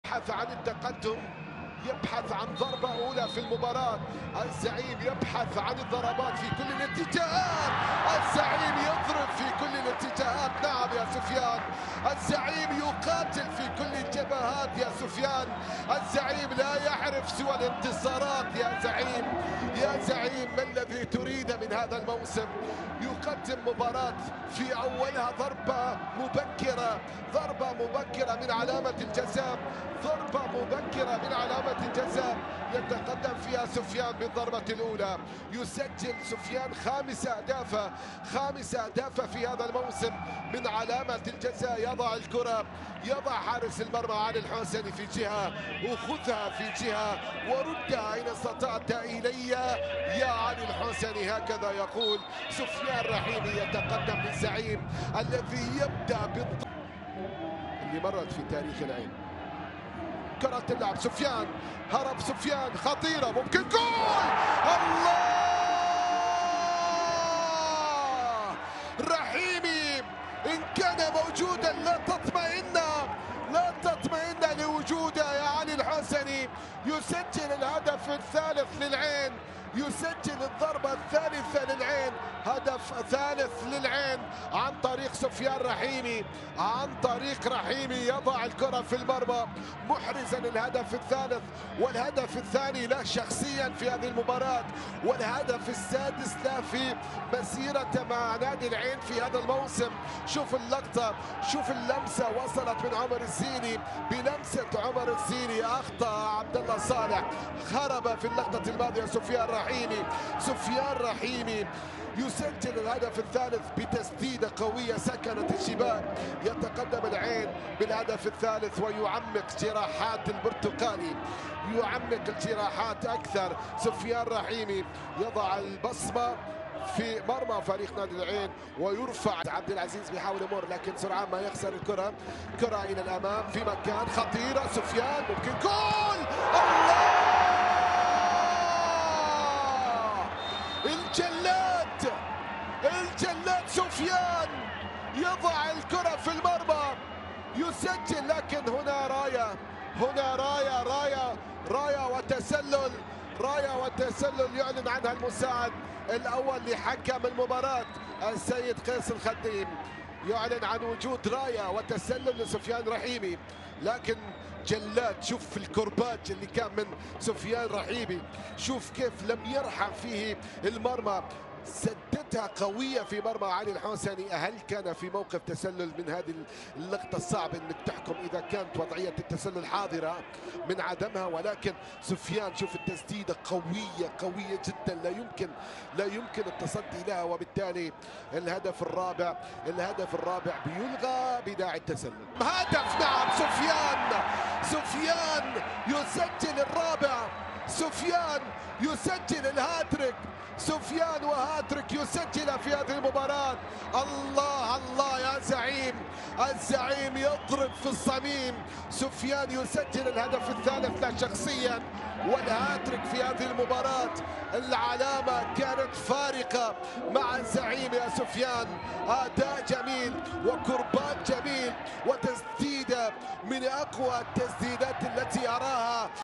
هاتريك سفيان رحيمي في مرمى عجمان
شاهد الهاتريك الرائع من النجم المغربي سفيان رحيمي في شباك نادي عجمان في الجولة الـ16 من دوري أدنوك للمحترفين